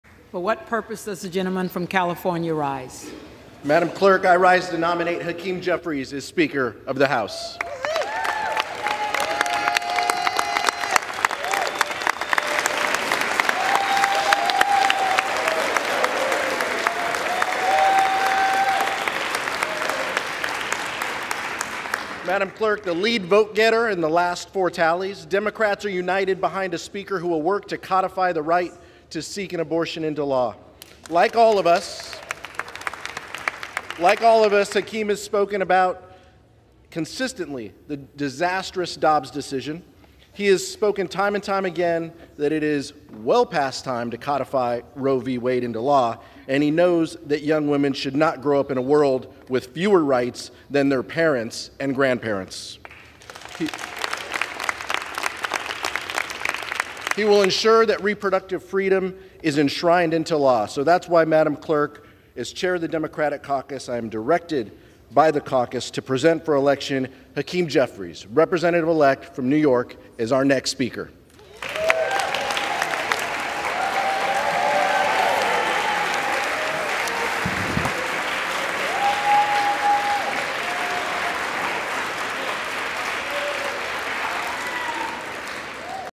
'The Lead Vote-Getter': Pete Aguilar Nominates Hakeem Jeffries To Be House Speaker
On the House floor Wednesday, Rep. Pete Aguilar (D-CA) spoke about Rep. Hakeem Jeffries (D-NY).